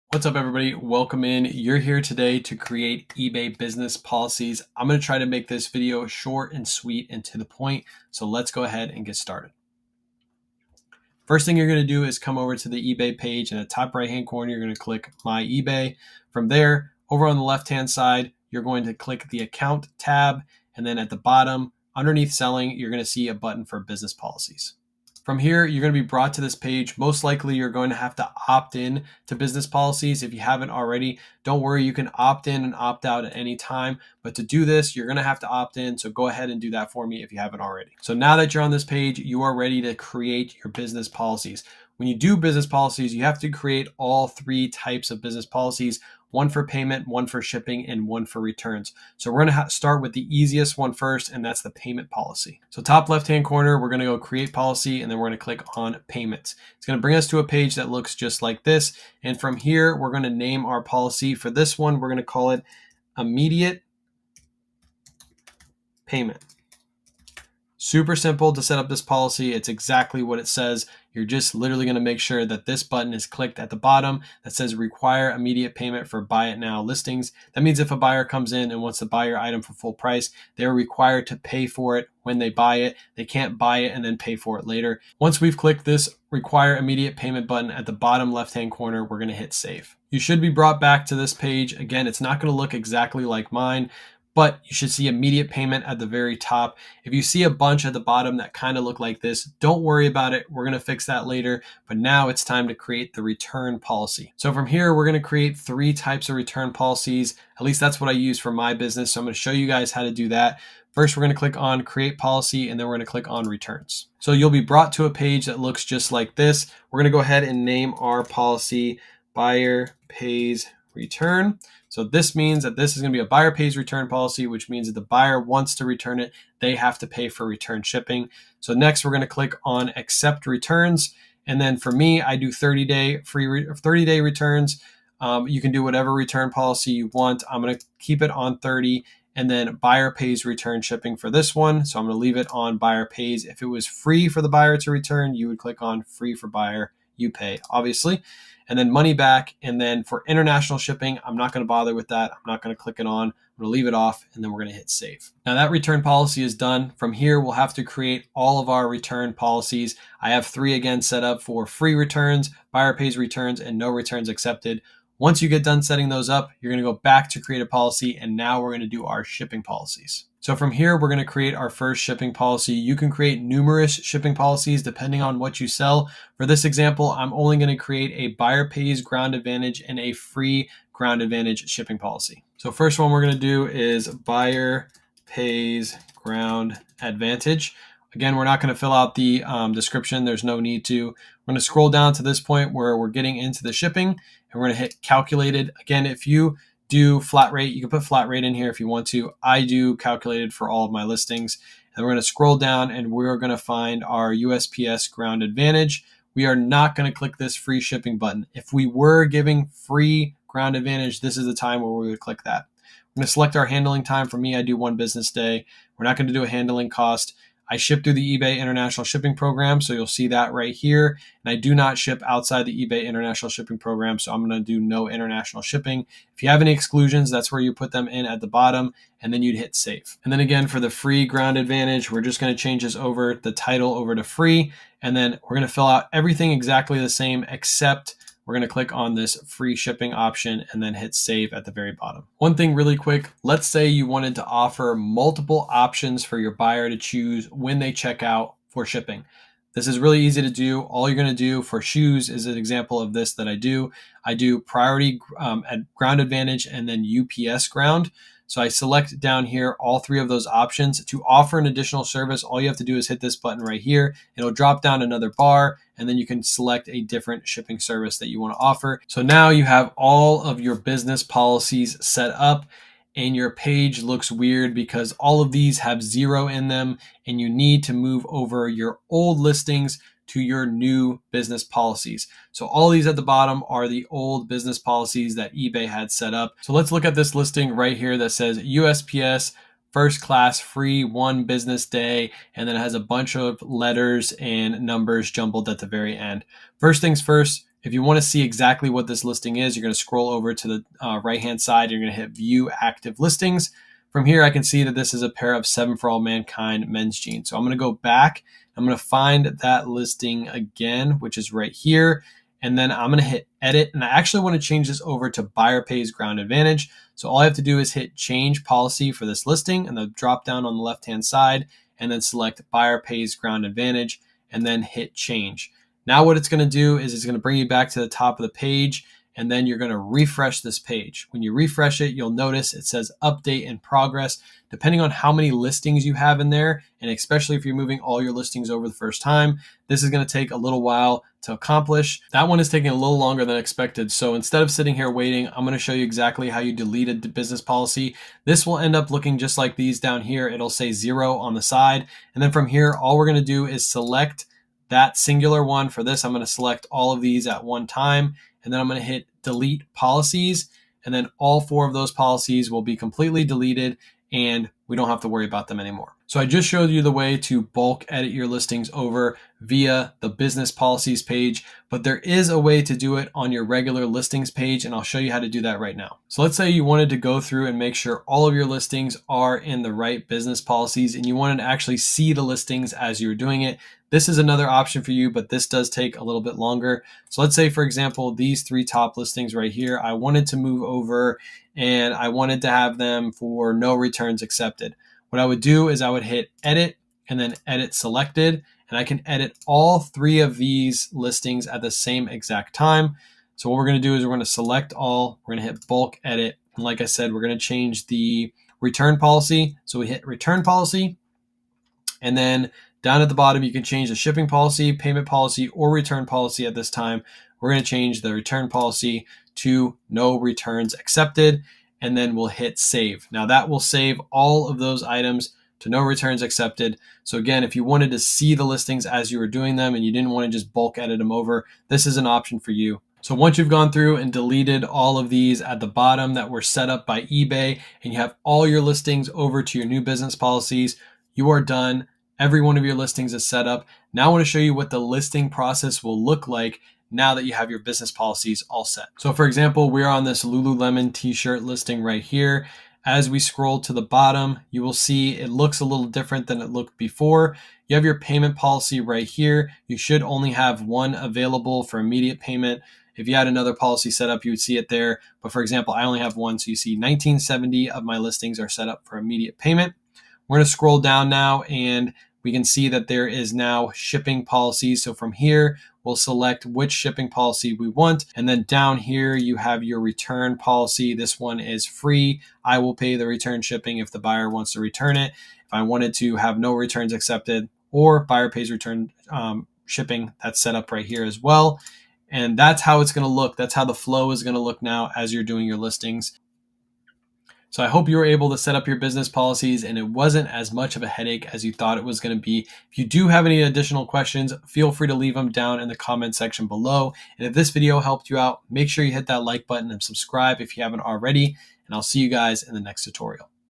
Quick Tutorial on to Set Up eBay Business Policies!
Quick guide on how to set up eBay Business Policies! If you have any questions, feel free to drop them in the comments section.